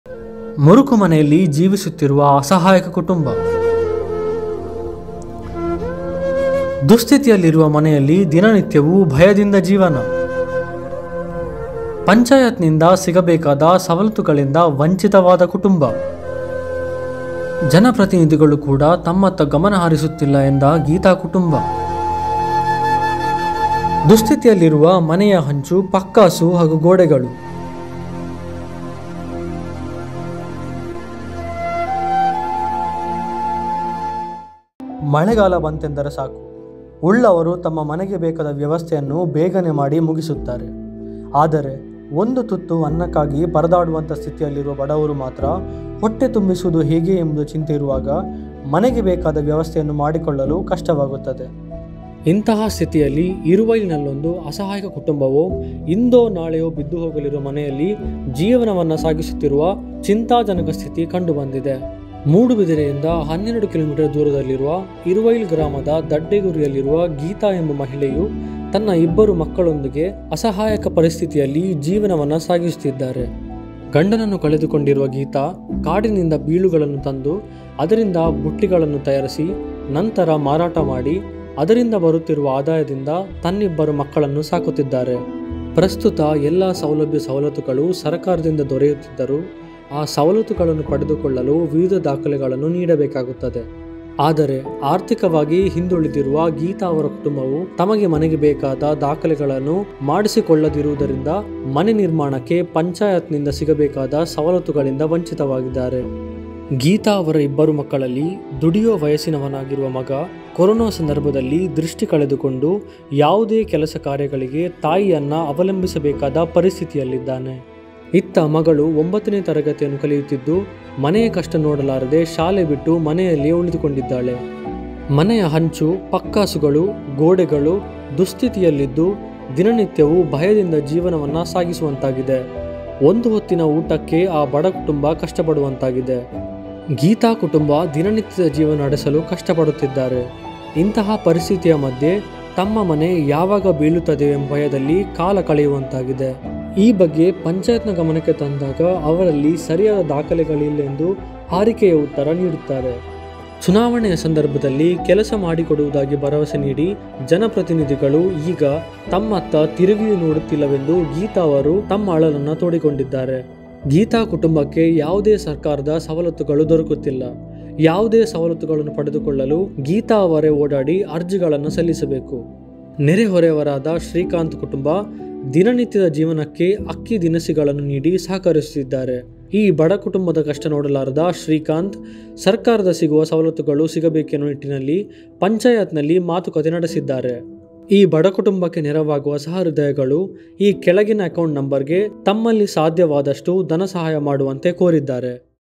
ಇರುವೈಲಿನ ಮುರುಕು ಮನೆಯೊಂದರಲ್ಲಿ ಜೀವಿಸುತ್ತಿದೆ ಅಸಹಾಯಕ ಬಡ ಕುಟುಂಬ | News Kannada
ಮೂಡಬಿದ್ರೆ: ಇರುವೈಲಿನಲ್ಲೊಂದು ಅಸಹಾಯಕ ಕುಟುಂಬವು ಇಂದೋ, ನಾಳೆಯೋ ಬಿದ್ದು ಹೋಗಲಿರುವ  ಮನೆಯಲ್ಲಿ ಜೀವನ ಸಾಗಿಸುತ್ತಿದೆ. ಮೂಡುಬಿದಿರೆಯಿಂದ 12 ಕಿ.ಮೀ ದೂರದಲ್ಲಿರುವ ಇರುವೈಲು ಗ್ರಾಮದ ದಡ್ಡೆಗುರಿಯಲ್ಲಿರುವ ಗೀತಾ ಎಂಬ ಮಹಿಳೆಯು ತನ್ನ ಇಬ್ಬರು ಮಕ್ಕಳೊಂದಿಗೆ  ಅಸಹಾಯಕ ಪರಿಸ್ಥಿತಿಯಲ್ಲಿ ಜೀವನವನ್ನ ಸಾಗಿಸುತ್ತಿದ್ದಾರೆ.

ಧನ ಸಹಾಯ ಮಾಡಲು ಇಚ್ಛಿಸುವವರು ಕೆಳಗಿನ ಬ್ಯಾಂಕ್‌ ಖಾತೆಗೆ ತಮ್ಮಲ್ಲಿ ಸಾಧ್ಯವಾದಷ್ಟು ಸಹಾಯ ಮಾಡಬೇಕಾಗಿ ವಿನಂತಿ.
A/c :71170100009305
IFSC: BARB0VJKUPP